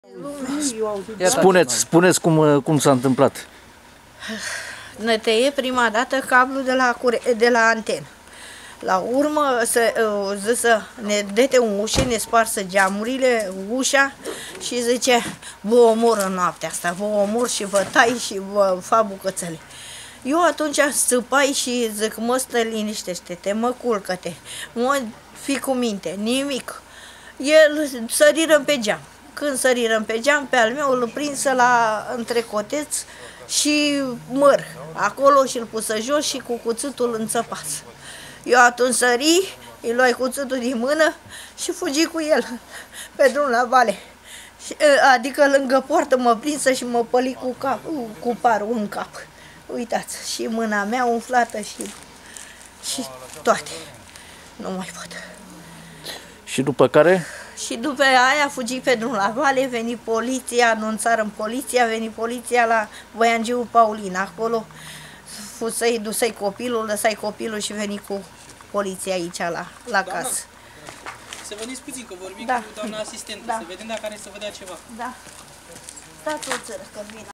DECLARATIA SOTIEI BĂRBATULUI INJUNGHIAT IN LOCALITATEA STEJARI-GORJ DE FIUL ACESTORA
DECLARATIA SOTIEI BĂRBATULUI INJUNGHIAT IN LOCALITATEA STEJARI-GORJ DE FIUL ACESTORA (Imagine: Ionel Cilica-DEACONU)